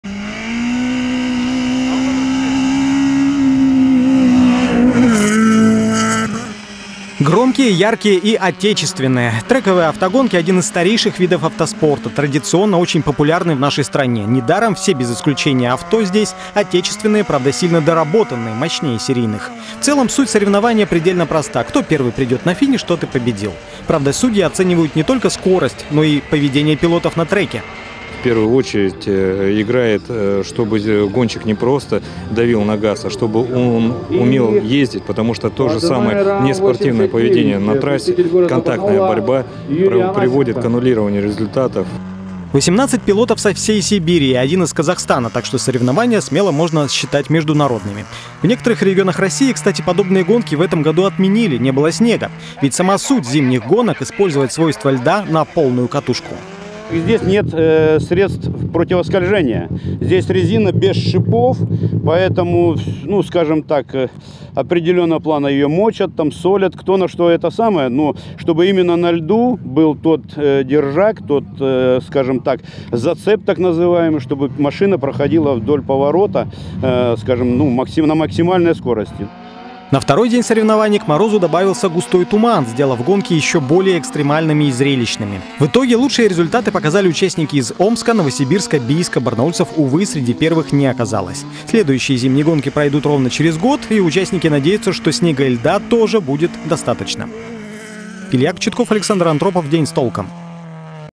Экстремальные заносы: зимние трековые гонки в Барнауле прошли при нулевой видимости
На барнаульском ипподроме прошел чемпионат Алтайского края по зимним трековым гонкам. Как прошли заезды, и кто стал лучшим - смотрите в нашем сюжете.